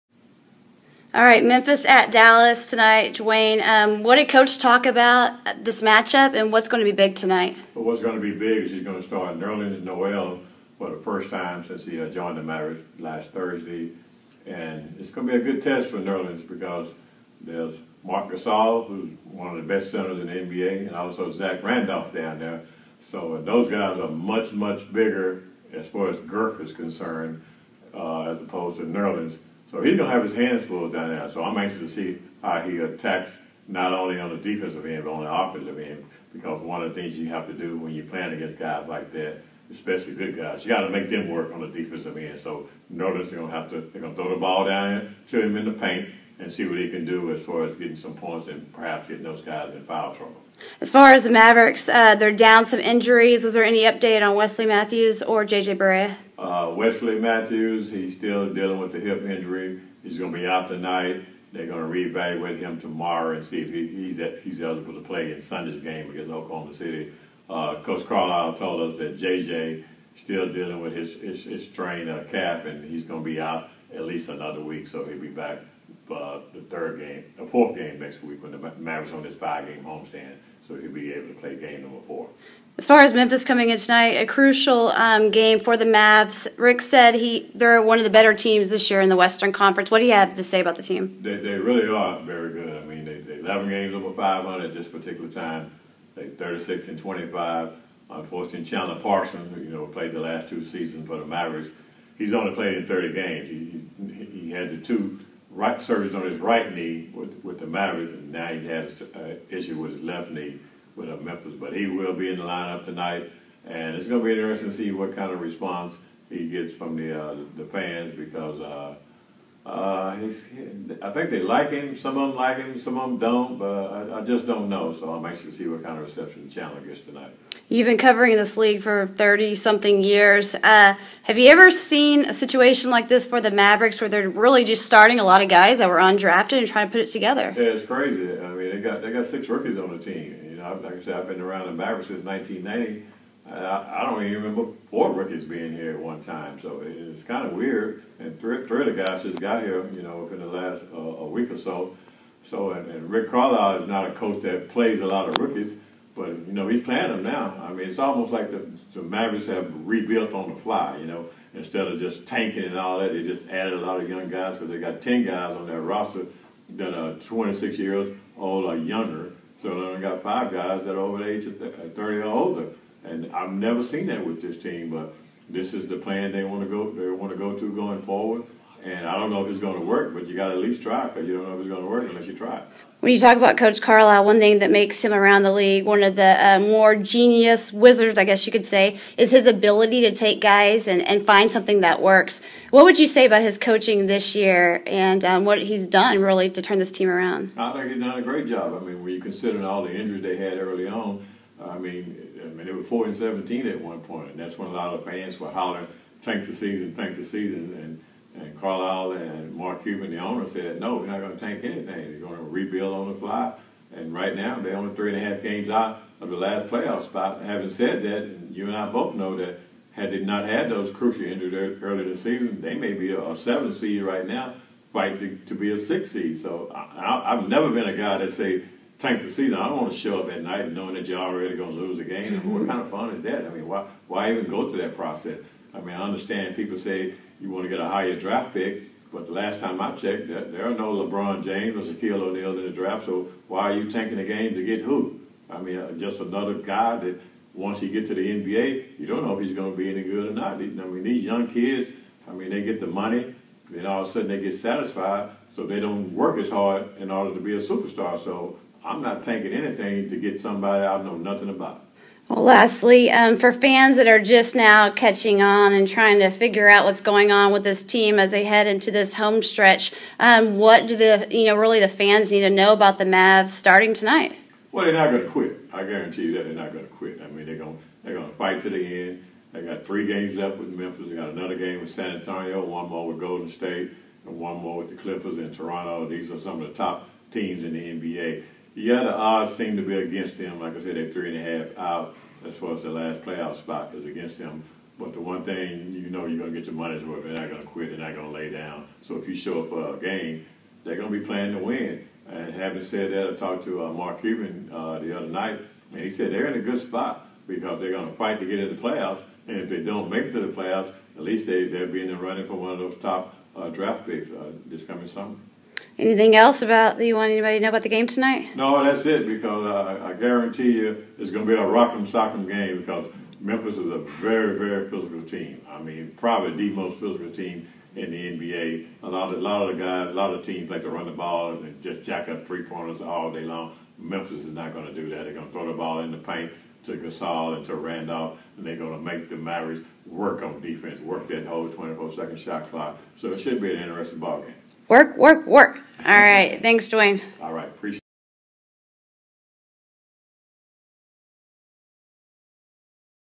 Dallas Mavericks Report
Updates on the Dallas Mavericks going into Friday's game against the Memphis Grizzlies